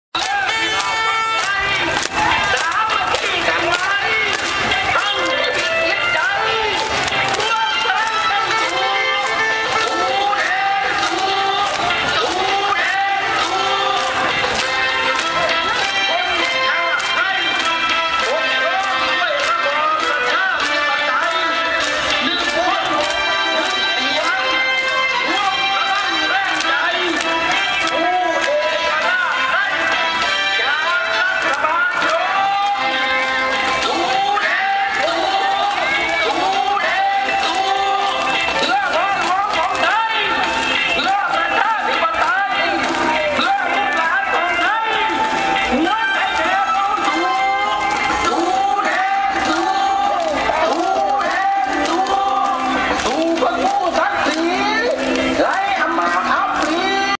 UDD protest in Bangkok
UDD protest in Bangkok. Thailand, 16/03/2010